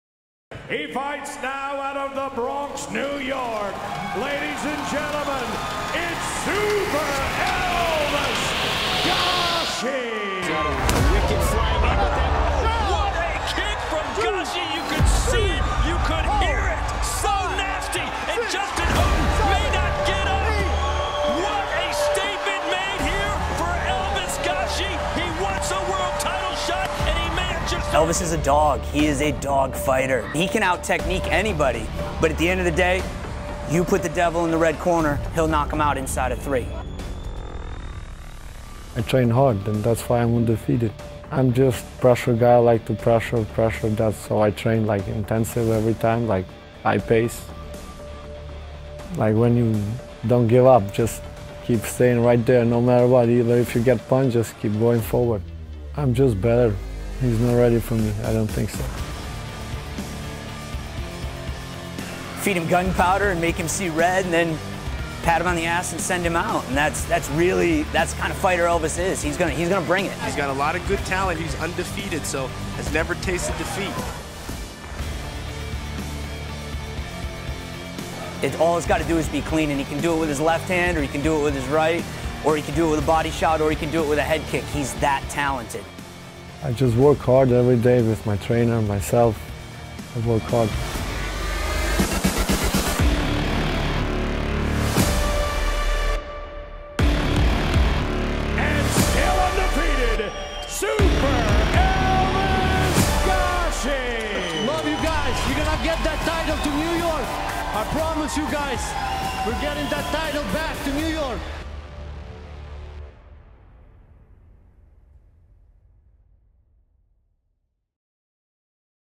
GLORY 73: Fighter Feature - Elvis Gashi
Elvis Gashi takes on Marat Grigorian for the lightweight championship at GLORY 73 SHENZHEN on December 7.